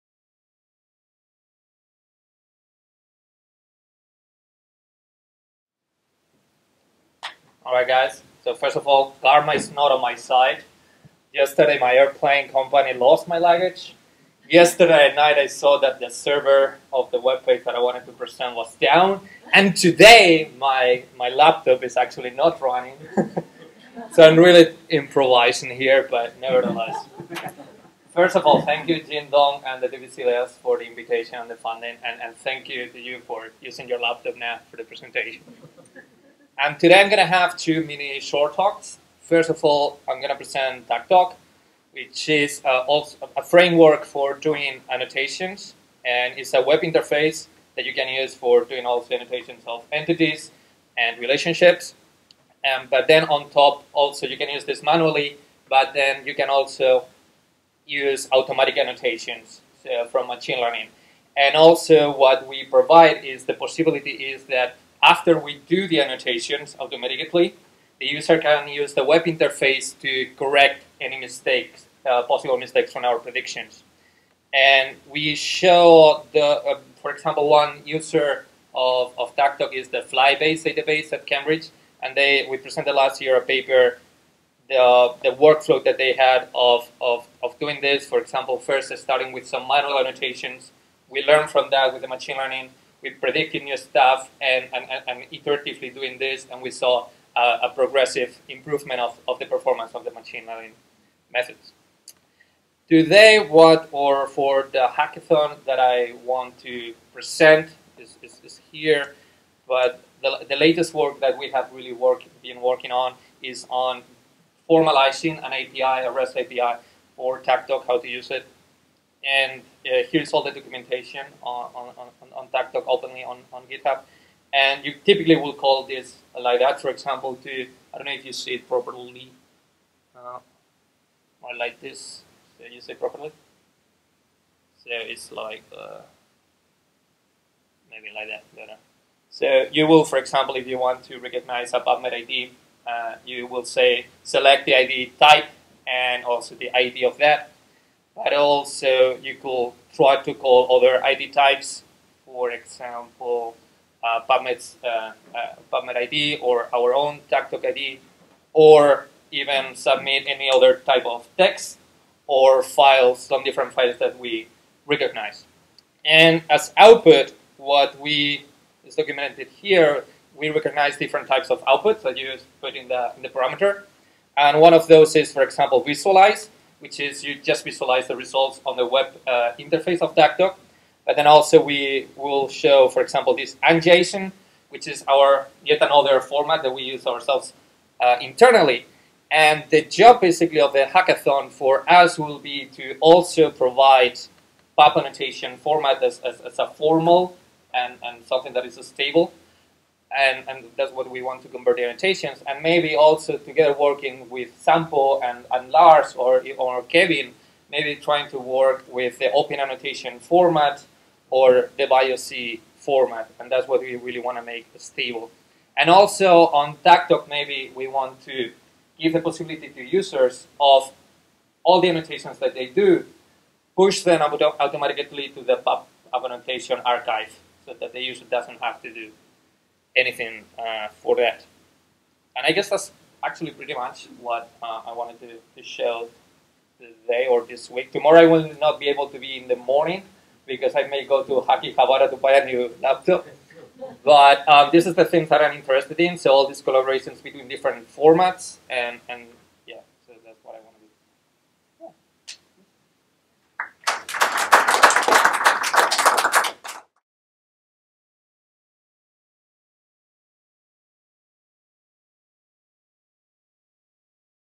Making tagtog interoperate with PubAnnotation
Biomedical Linked Annotation Hackathon (BLAH) 2 was held in DNA Data Bank of Japan, National Institute of Genetics in Mishima, Shizuoka, Japan. On the first day of the Hackathon (16. Nov.), public symposium of the BLAH 2 was held.

In this talk, Juan Miguel Cejuela makes a presentation entitled "Making tagtog interoperate with PubAnnotation". (4:25)